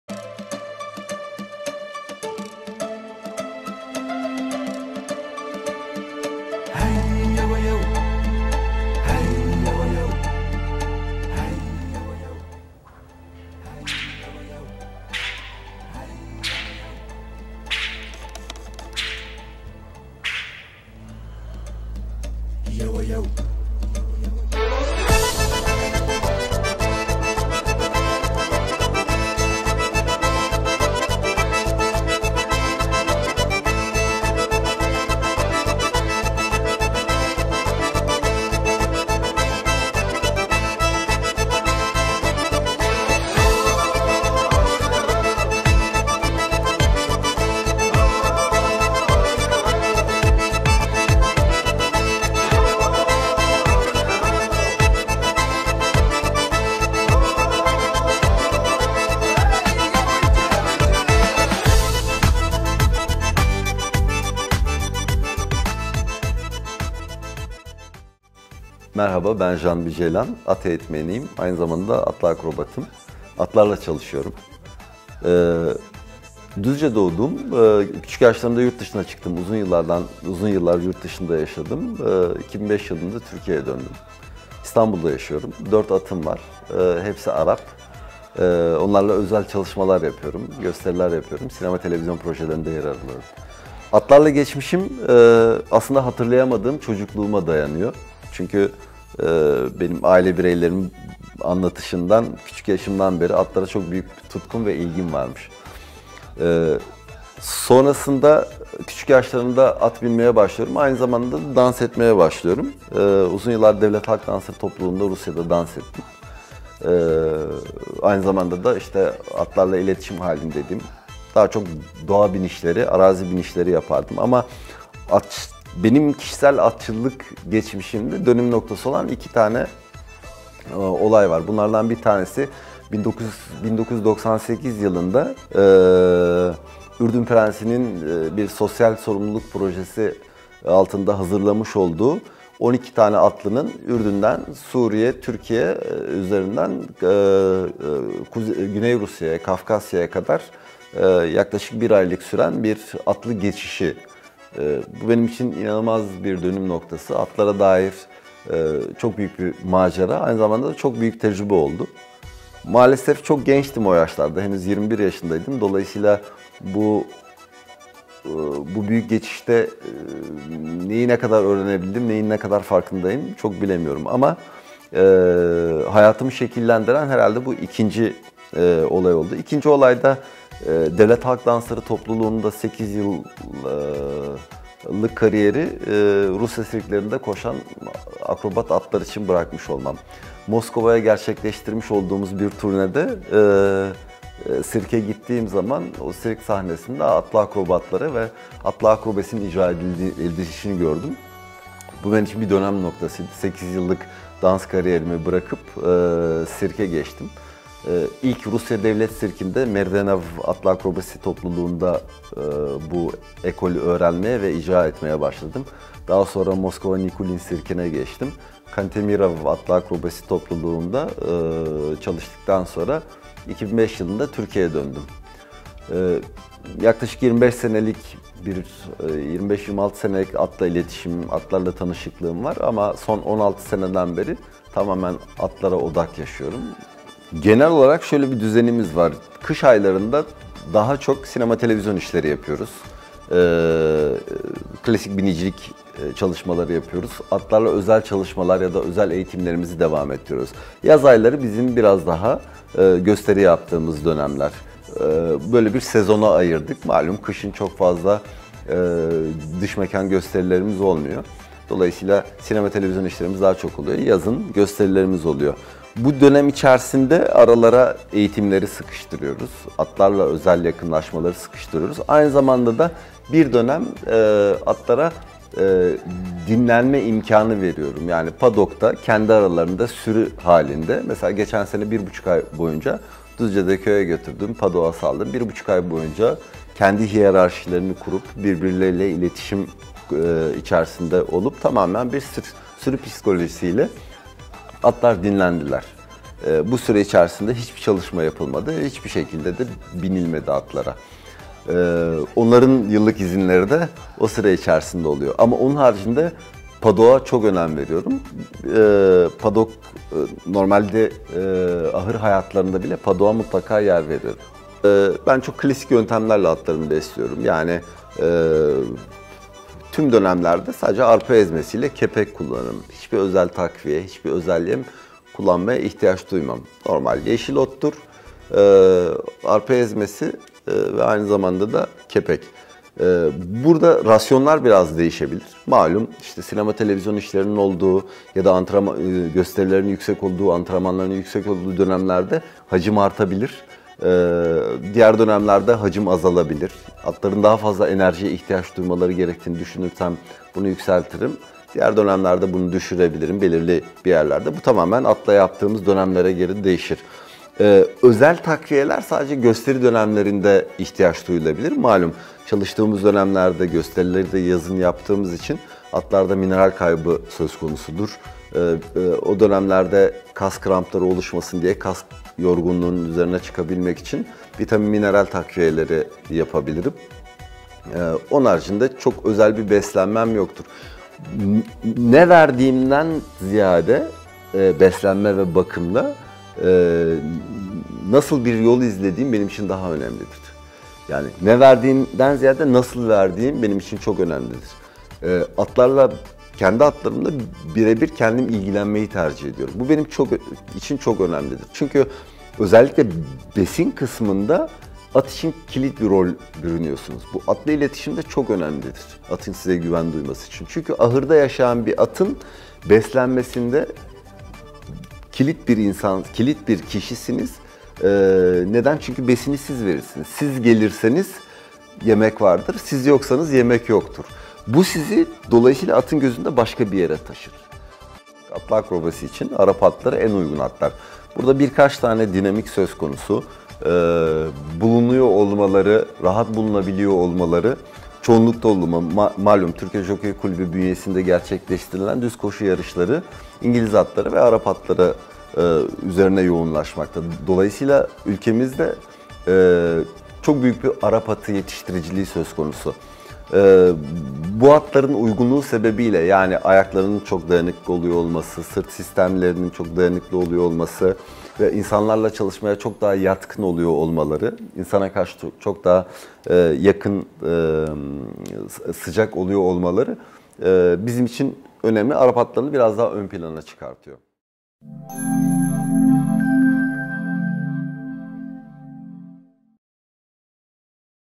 Janbi Ceylan - At Eğitmeni ve Atlı Akrobat
At Eğitmeni ve Atlı Gösteri Akrobatı Janbi Ceylan ile atlar üzerine gerçekleştirdiğimiz söyleşinin detaylarına dergimizin Şubat sayısında ulaşabilirsiniz.